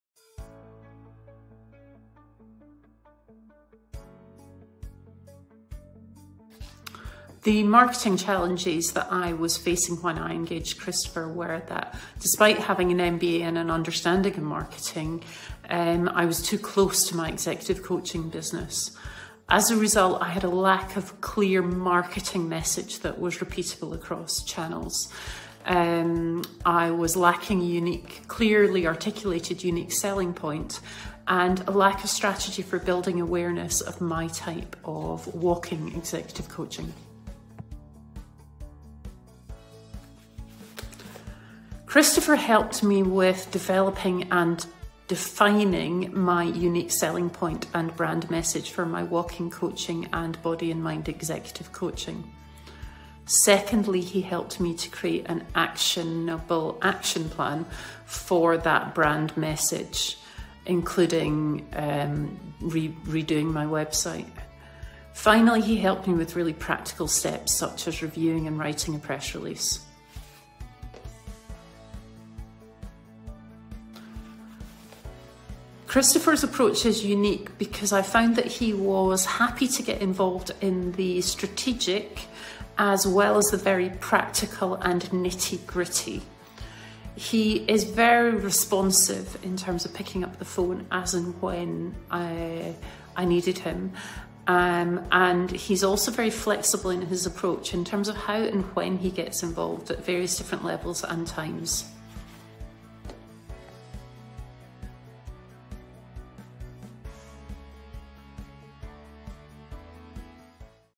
Complete Leader Executive Coaching - Real Marketing Video Testimonial
The Complete Leader is an executive and leadership coaching business that stands out through its focus on 'body as well as mind'.

Real Marketing provided marketing strategy & planning, a new sales pitch, press release editing, new website content & project management.

The business's founder, Maureen Evans, says: "... Christopher helped with defining my USP and brand message, created an actionable action plan... and took really practical steps... He's happy to get involved in the strategic as well as the practical and nitty gritty. He's very responsive in terms of picking up the phone and he's very flexible in his approach about how and when he gets involved..."

You can learn about Real Marketing's comprehensive, holistic marketing consultant services for small and medium sized businesses